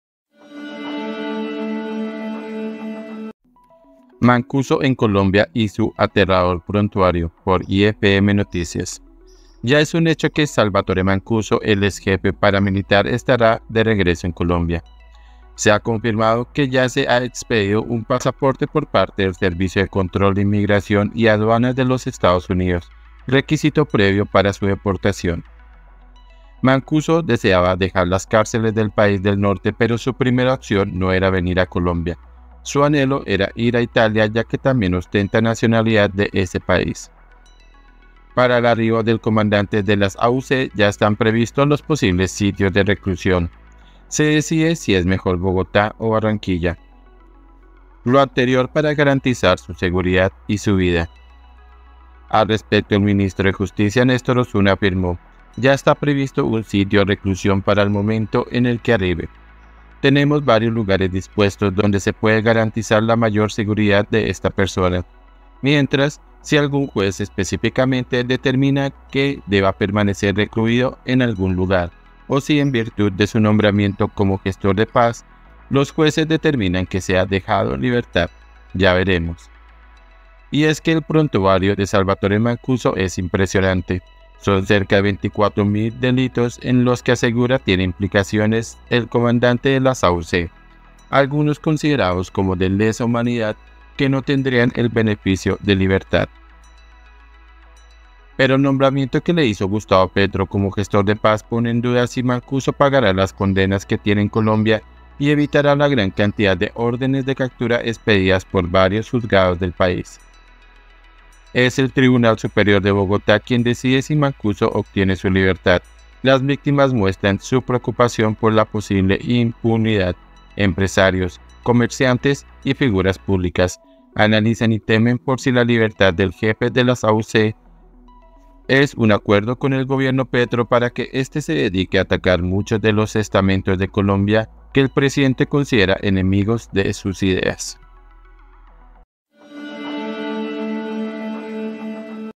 ⛔ MANCUSO EN COLOMBIA Y SU ATERRADOR PRONTUARIO
Pero el nombramiento que le hizo Gustavo Petro como “gestor de paz”, pone en duda si Mancuso pagará las condenas que tiene en Colombia y evitará la gran cantidad de órdenes de captura expedidas por varios juzgados del país.
IFM NOTICIAS